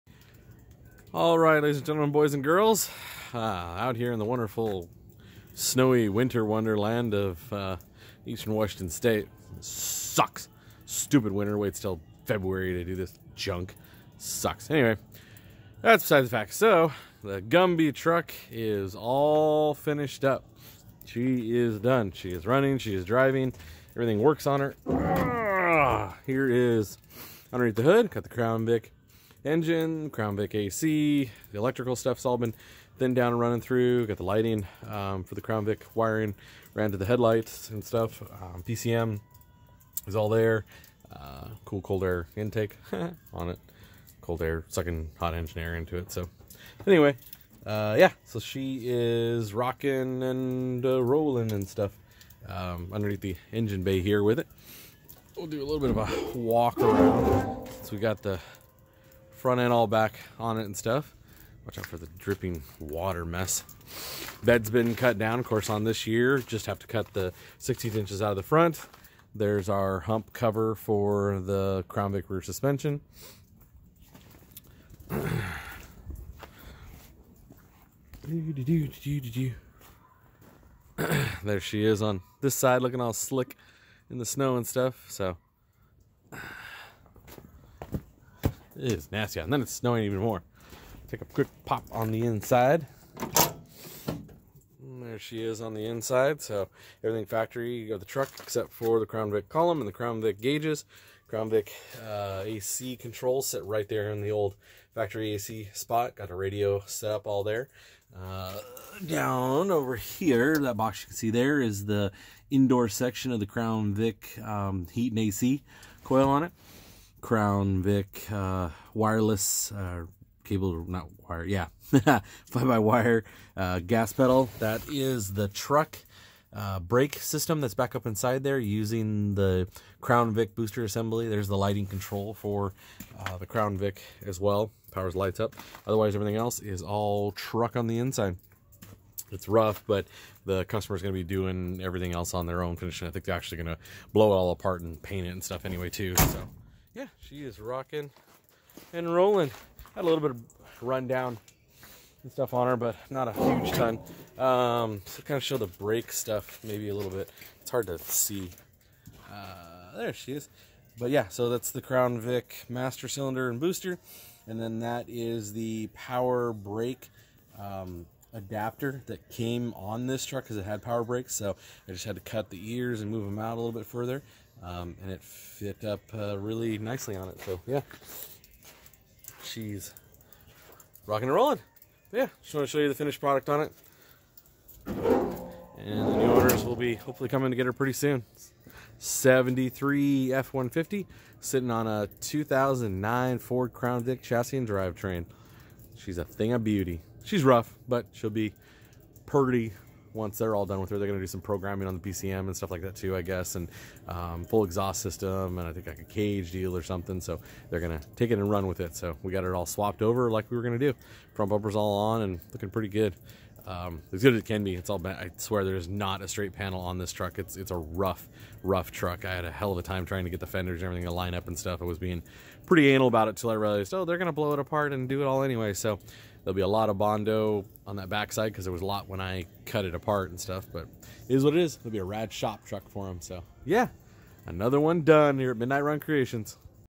1978 f150 and crown vic full chassis swap is complete
Gumby. the 78 f150 on a 2009 ford crown victoria chassis and drive train is all fished up as far as we are taking it. its a disk tire burnning fun machine.